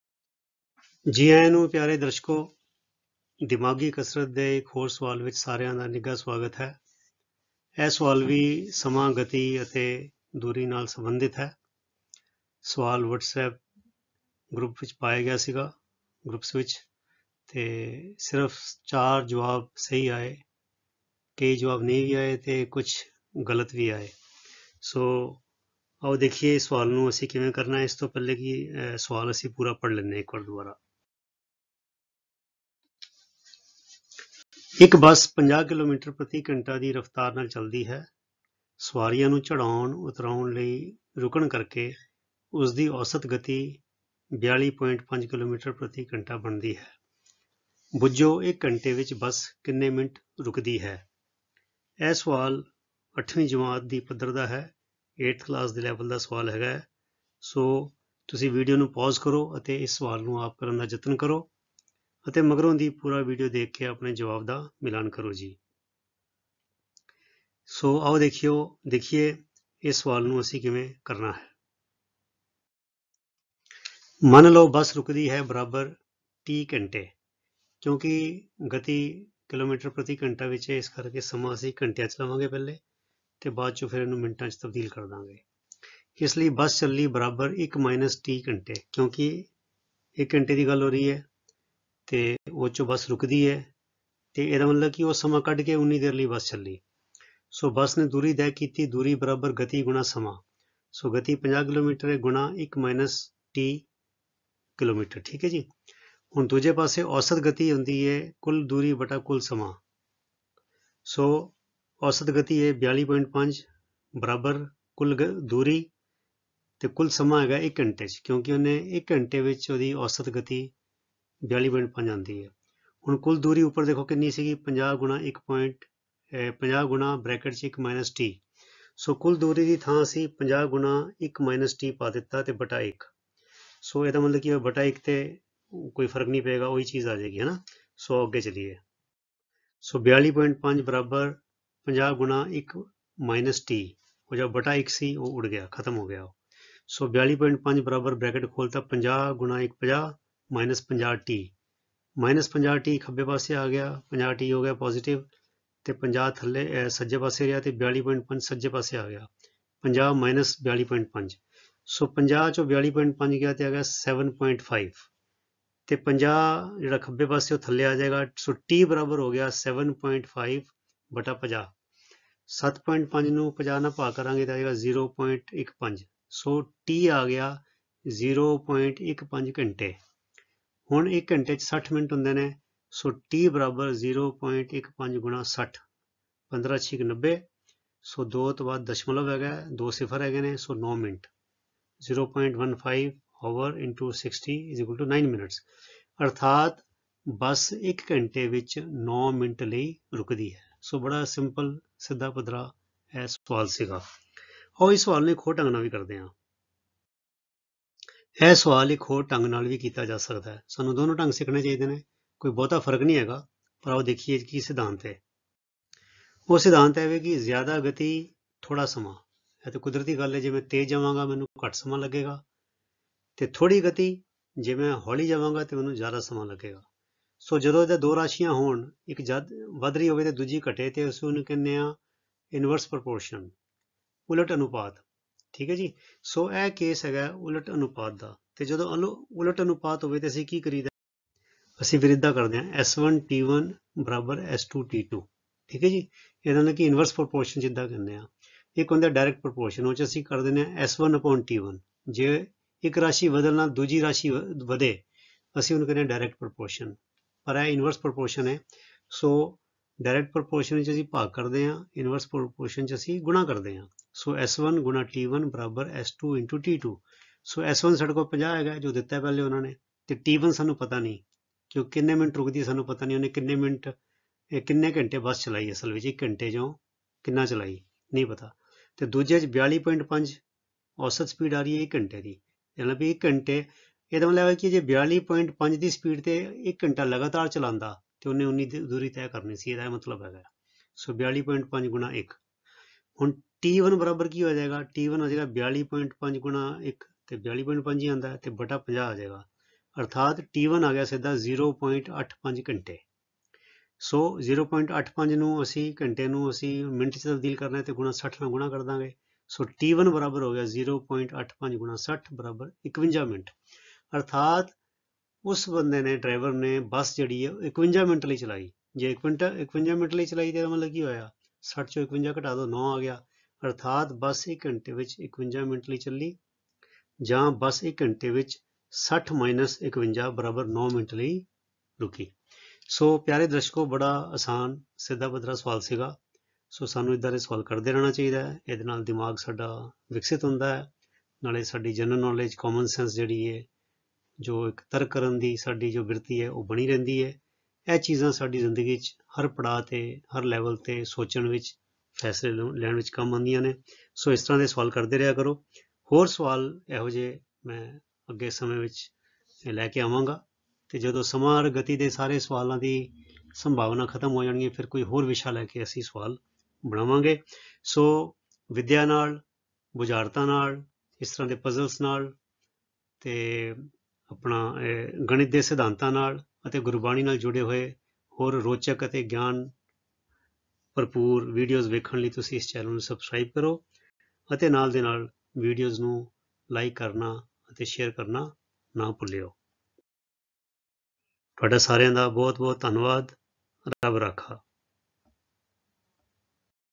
ਬਸ ਕਿੰਨੇ ਮਿੰਟ ਲਈ ਰੁਕੀ ? How much does the bus stop in one hour ?
ਸਮਾਂ, ਗਤੀ ਅਤੇ ਦੂਰੀ ਦਾ ਸਵਾਲ Speed, time and distance question.